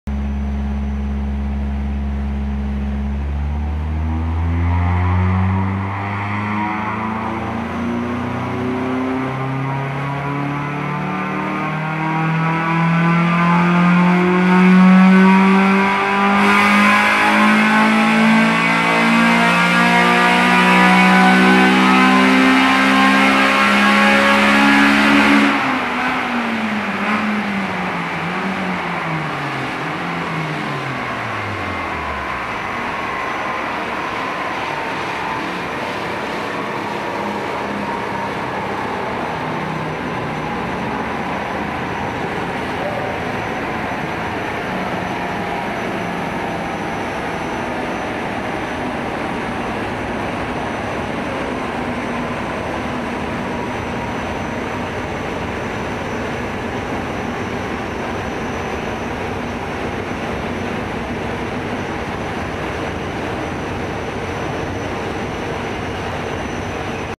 Honda Dyno Day at Sportchip
third Honda Run at Sportchip Unit1 , Primesdie Park, Ballycoolin Dublin15